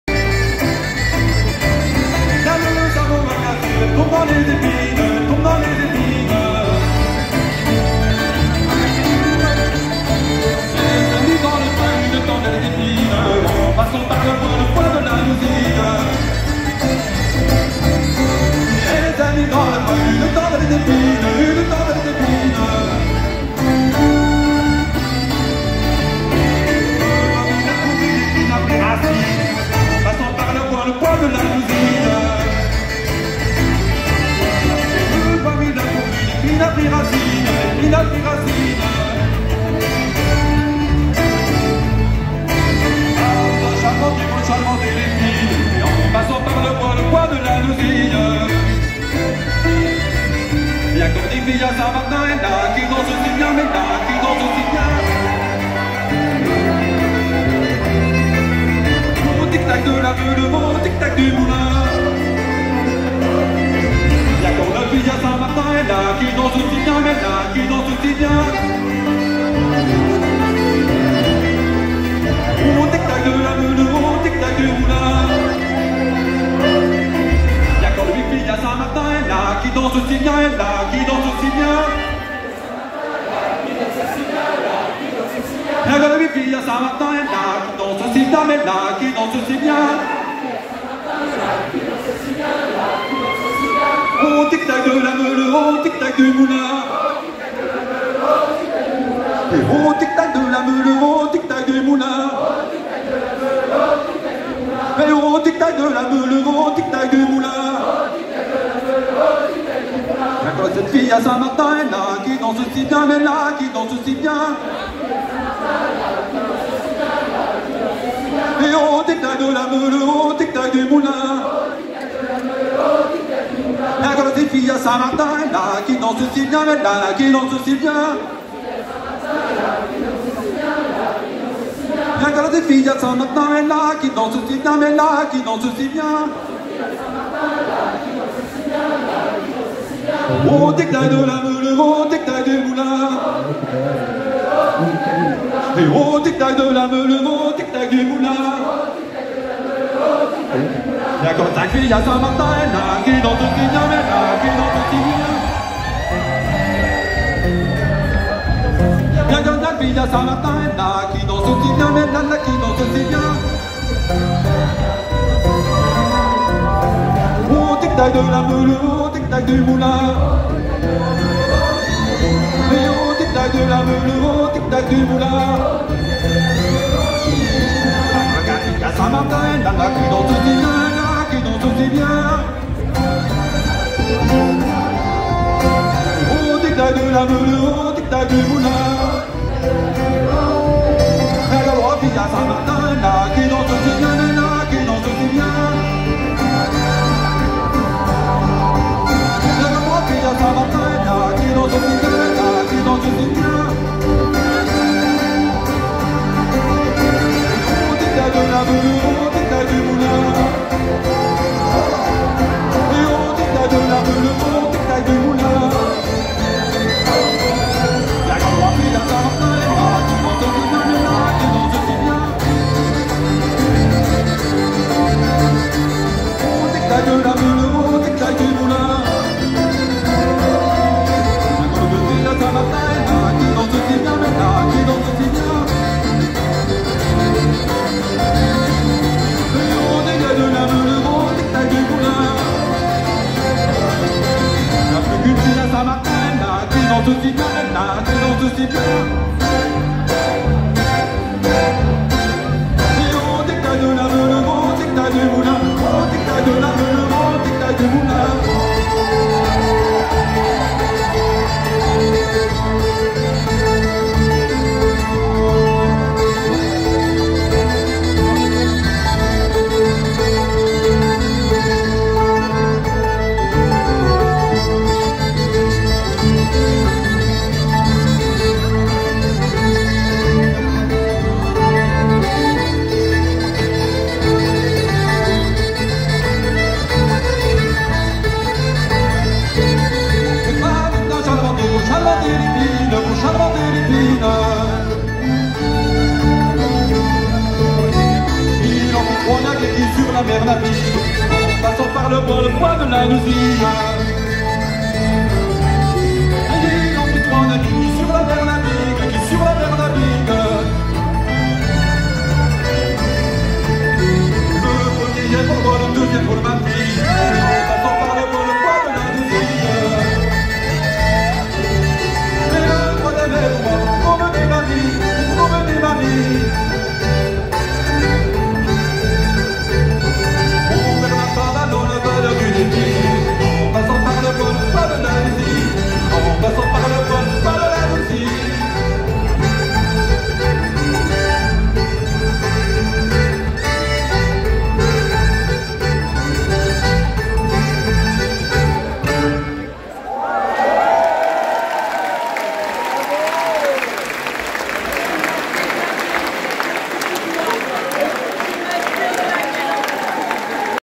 Hamon Martin 5tet / Rond St Vincent / Roue Waroch Plescop 2023
Le groupe Hamon Martin Quintet avec un rond de Saint Vincent lors du fest-noz du festival Roue Waroch de Plescop 2023.

Crédit Audio : Hamon Martin 5tet / Crédit Vidéo : Norbert Fest Noz